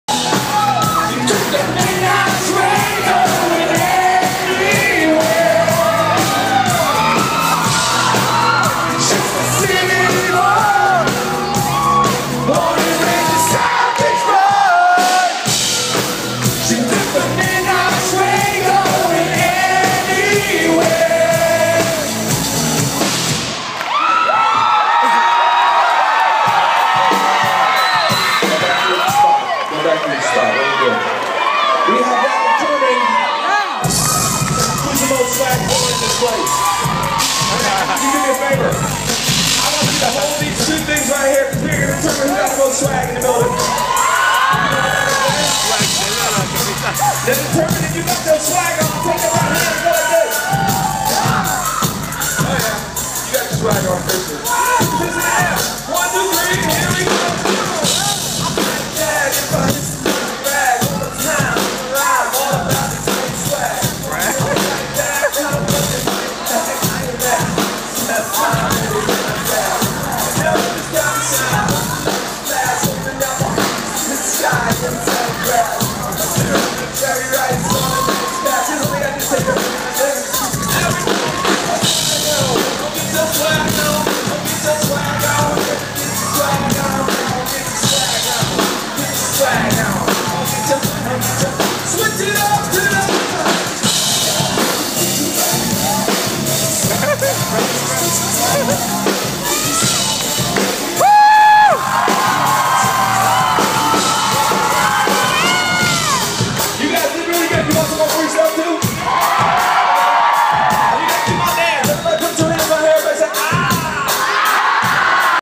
Air1s Brant Hansen club awesome Phoenix
Brant Hansen singing and kj-52 with some swag